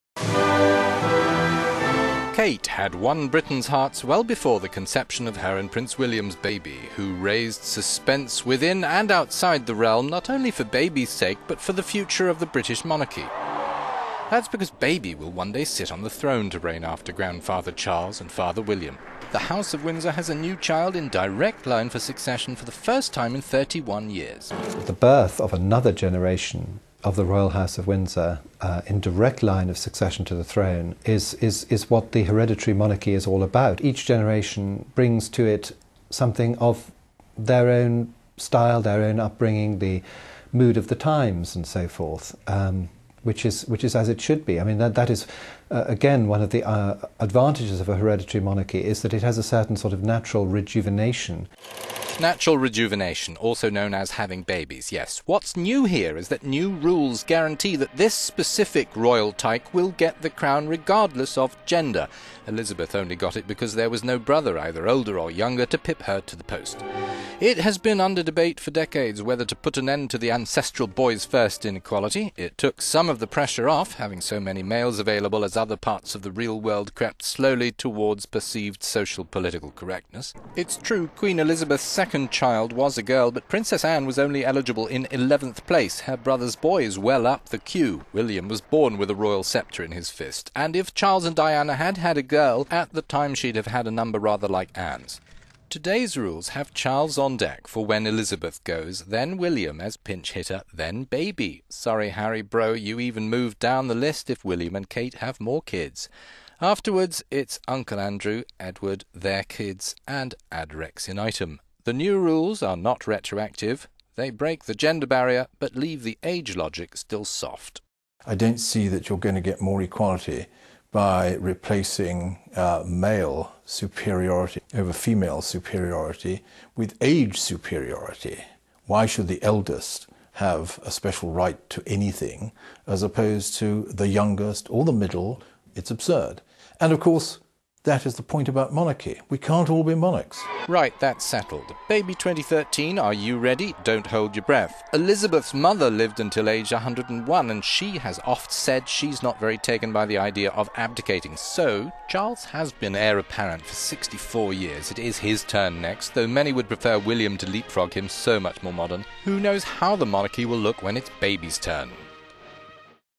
Hey Baby! Reign over Britain one day, gender no object
Kate had won Britons' hearts well before the conception of her and Prince William's baby, who...

Kate had won Britons' hearts well before the conception of her and Prince William's baby, who raised suspense within and outside the realm, not only for Baby's sake but the future of the British monarchy.

That's because Baby will one day sit on the throne, to rein after Grandfather, Charles and Father, William. The House of Windsor has a new child in direct line for succession for the first time in 31 years.

Hugo Vickers, Royal historian, said: "The birth of another generation of the Royal House of Windsor, in direct line of succession to the throne, is what the hereditary monarchy is all about. Each generation brings to it something of their own style, their own upbringing, their own mood of the times and so forth, which is as it should be. That is one of the advantages of a hereditary monarchy, that it has a sort of natural rejuvenation."

'Natural rejuvenation' also known as 'having babies', yes.

What's changed here is that new rules guarantee that this specific royal tyke will get the crown regardless of gender. Elizabeth II only got it because there was no brother, either older or younger, to pip her to the post.

It has been under debate for decades whether to put an end to the ancestral 'boys first' inequality. It took some of the pressure off having so many males available as other parts of the real world crept slowly towards perceived social-political correctness.

Queen Elizabeth's second child was a girl, but Princess Anne was only eligible in eleventh place, her brother's boys well up the queue. William was born with a royal sceptre in his fist. And if Charles and Diana had had a girl, at the time she'd have had a number rather like Anne's.

Today's rules have Charles on deck for when Elizabeth goes, then William as pinch hitter, then Baby! Harry even moves down the list if William and Kate have more kids. Afterwards it's Uncle Andrew, Edward, their kids and so on.

The new rules are not retroactive.

They break the gender barrier but leave the age logic still soft.

Charles Mosley, 'royal author', said: "I don't see that you are going to get more equality by replacing male superiority over female superiority with age superiority. Why should the eldest have a special right to anything as opposed to the youngest or the middle? It's absurd. And of course that's the point about monarchy, we can't all be monarchs."

Baby 2013, are you ready?

Don't hold your breath. Elizabeth's mother lived until age 101, and she is not fond of the idea of abdicating. Sooo.